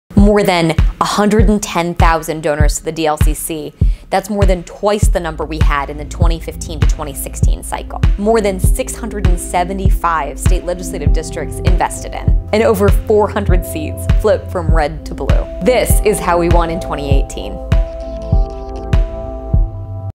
How We Won in 2018
Between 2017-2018, The DLCC helped flip more than 400 state legislative seats from red to blue. This is how we got here and where we’re going next